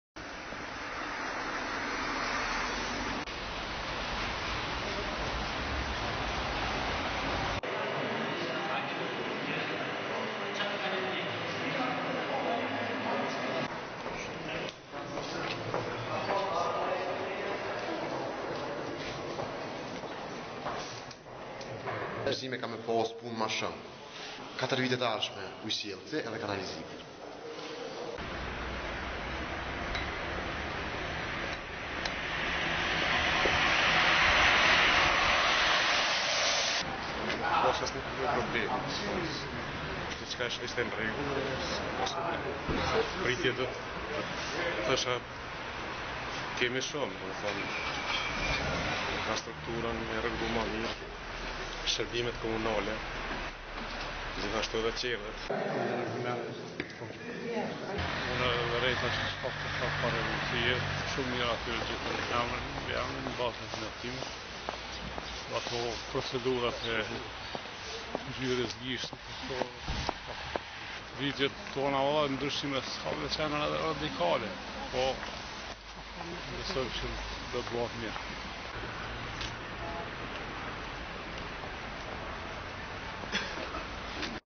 Gara për balotazh
Në 25 komuna të Kosovës, sot zhvillohet gara e balotazhit përmes së cilës qytetarët me të drejtë vote, kanë mundësinë që të votojnë për zgjedhjen e kryetarëve të rinj për mandatin e ardhshëm.